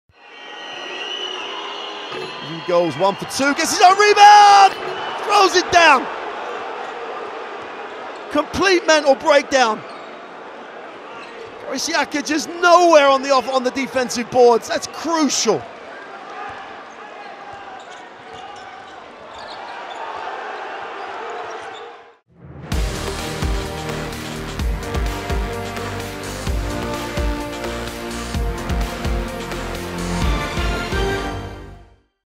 Melvin Ejim - Miss & Dunk in one action
Umana Reyer Venezia's Melvin Ejim missed the free-throw but shows a sensational reaction and scores the rebound with a dunk against Pinar Karsiyaka.